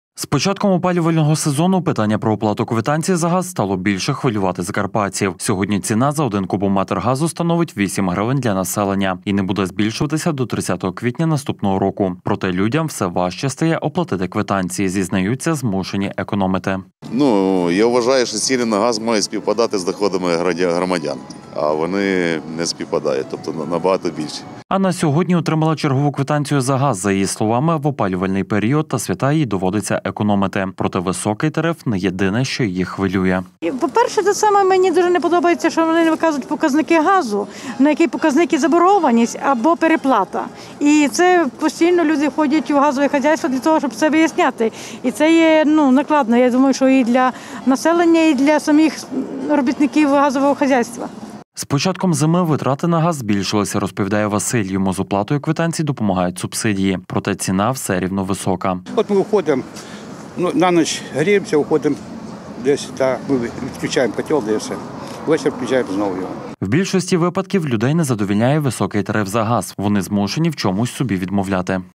А що з ціною на газ? Економія понад усе
Питання ціни на газ завжди було особливо болючим для населення та підприємців. Чи відповідає така вартість блакитного палива його якості. Та чи задоволені краяни роботою Закарпатгазу? З початком опалювального сезону питання про оплату квитанцій за газ стало більше хвилювати закарпатців. Сьогодні ціна за один кубометр газу становить 8 гривень для населення і не буде збільшуватись до 30 квітня 2022 року. Проте, людям все важче стає оплатити квитанції – зізнаються, змушені економити.  Анна сьогодні отримала чергову квитанцію за газ. За її словами, в опалювальний період та свята їй доводиться економити. Проте, високий тариф – не єдине, що її хвилює.  З початком зими витрати на газ збільшились, розповідає Василь. Йому з оплатою квитанцій допомагають субсидії. Проте, ціна все рівно висока, змушений економити. В більшості випадків людей незадовільняє високий тариф за газ. Вони змушені в чомусь собі відмовляти.